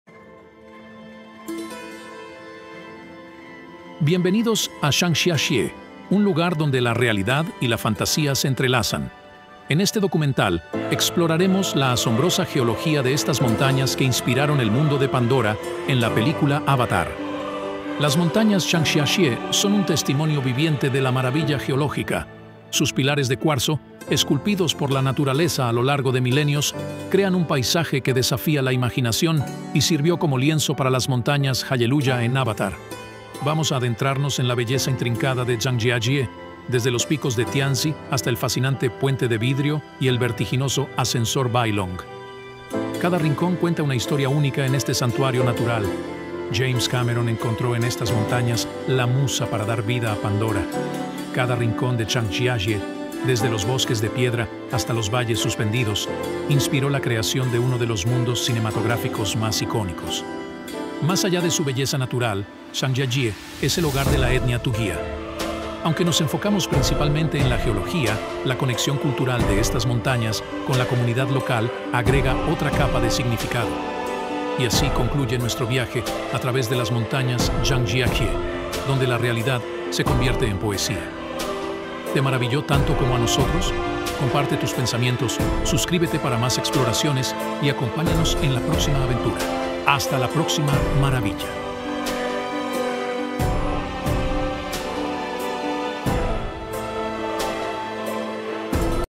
Zhangjiajie: Explorando las Montañas que Inspiraron Avatar | Geología Única y Belleza Natural 🏞️✨
Embárcate en un viaje fascinante a través de las Montañas Zhangjiajie, la auténtica inspiración detrás del épico paisaje de Pandora en Avatar. Descubre la geología única de estos pilares de cuarzo, explora puntos de interés como el Puente de Vidrio y el Ascensor Bailong, y sumérgete en la conexión directa entre la realidad de Zhangjiajie y la fantasía de Avatar. Un documental que desentraña los secretos de este santuario natural. ¡No te pierdas esta exploración única de la Tierra Real que cautivó a James Cameron! 🌄🌿